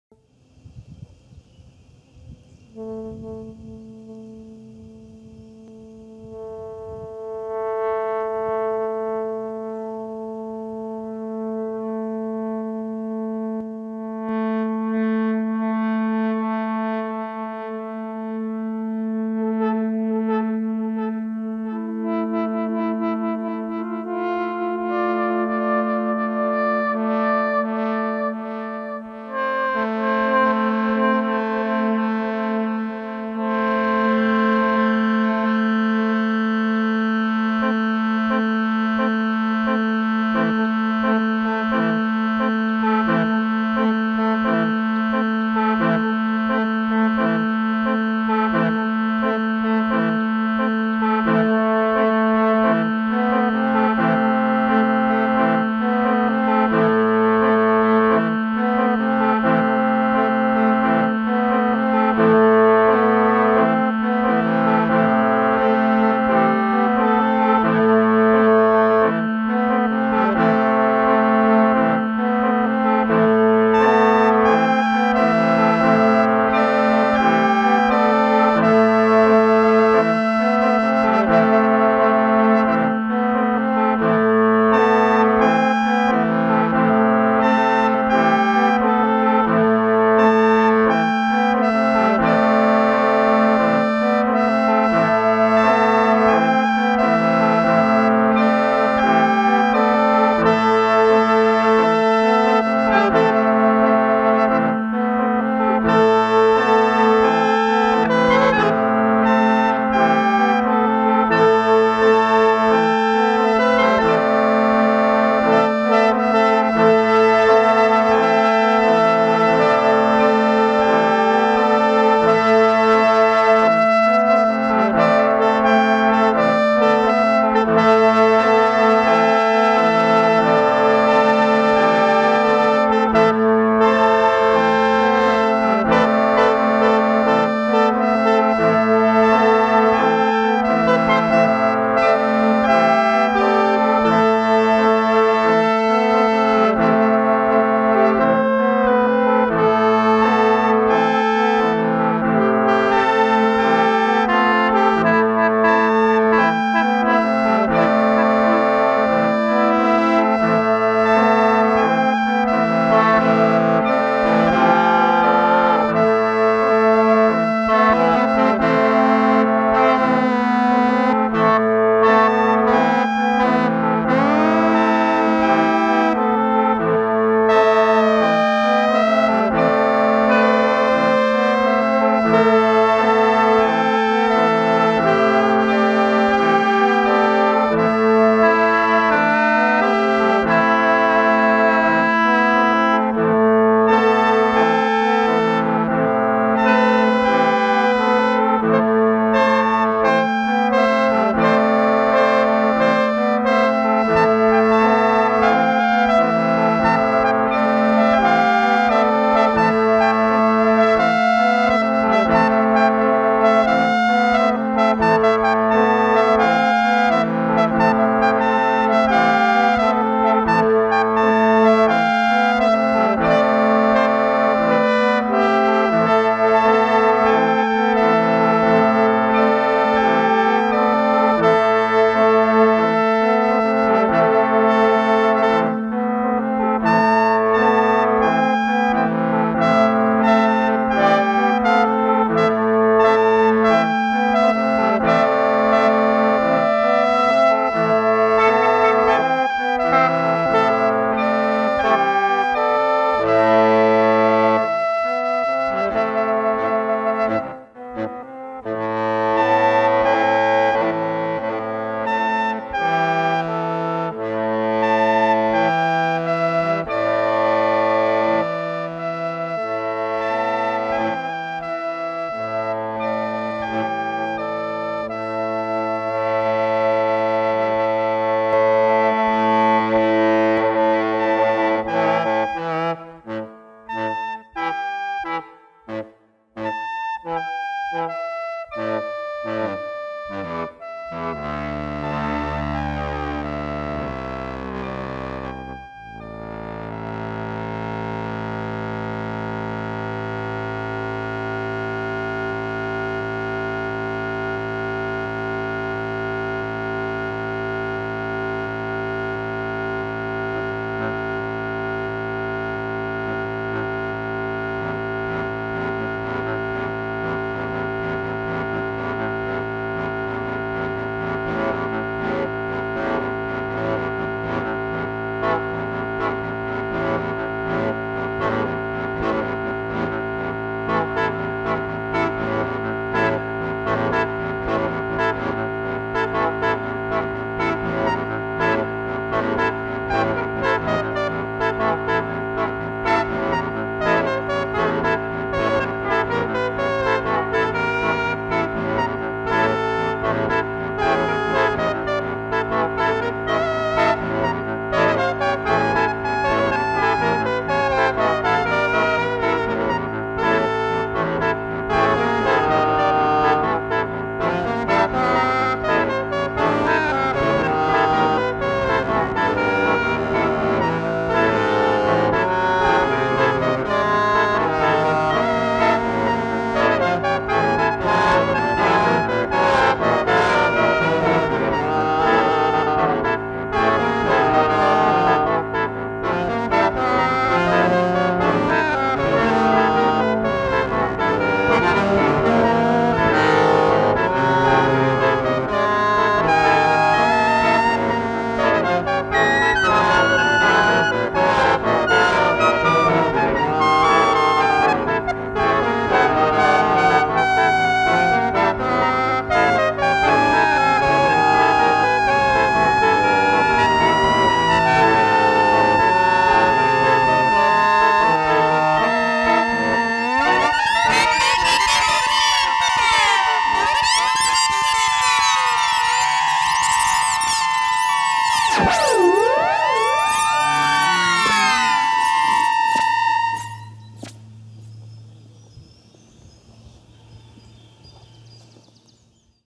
Roli & Axoloti afternoon in the grass #2
an improvised jam using a Roli Seaboard Block connected to an Axoloti Synth.
The Axoloti patch is producing a 6 voice polyphonic MPE wavetable synth mixed into a 4 tracks "tape type" delay.